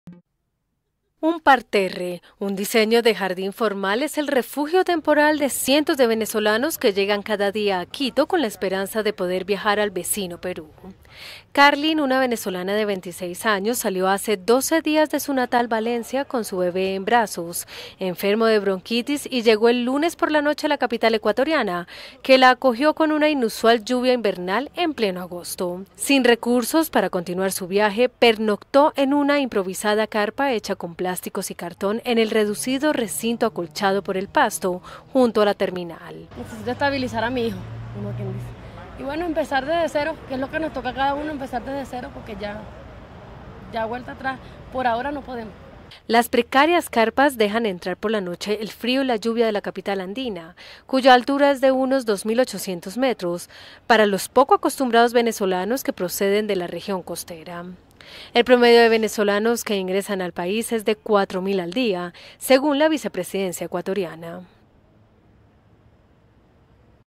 Parterre en terminal de Quito, parada de cientos de venezolanos que quieren llegar a Perú
Quito, 1 ago (EFE), (Imagen: Juan Francisco Chávez).- Un parterre de 100 metros cuadrados frente a la terminal de autobuses de Carcelén, un barrio del norte de Quito, es el refugio temporal de cientos de venezolanos que llegan cada día con lo puesto y la esperanza de poder viajar al vecino Perú.
Carlin, una venezolana de 26 años, salió hace doce días de su natal Valencia con su bebé en brazos enfermo de bronquitis y llegó el lunes por la noche a la capital ecuatoriana, que la acogió con una inusual lluvia invernal en pleno agosto.